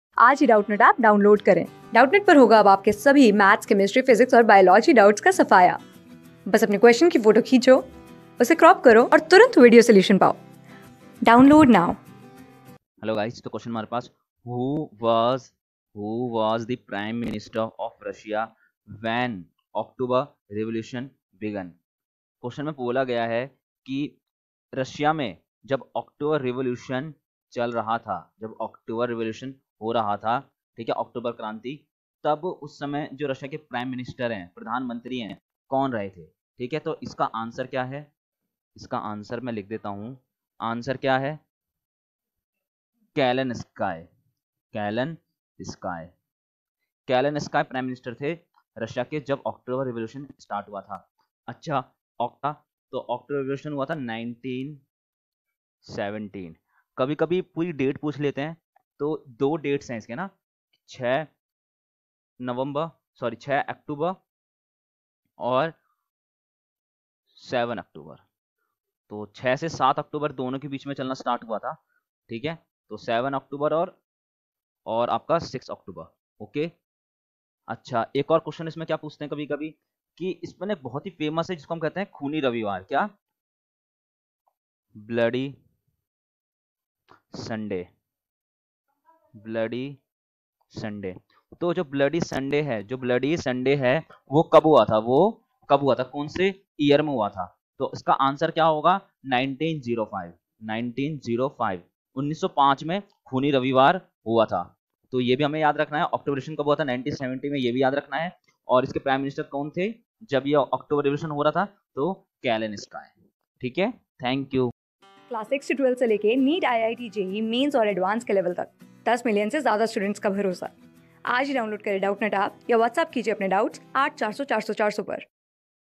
Who was the Prime Minister of Russia when October Revolution began? | CLASS 9 | SOCIALISM IN EUR...
Who was the Prime Minister of Russia when October Revolution began?
Class: 9
Subject: HISTORY
Chapter: SOCIALISM IN EUROPE AND THE RUSSIAN REVOLUTION
Board:CBSE

👉 Have Any Query? Ask Us.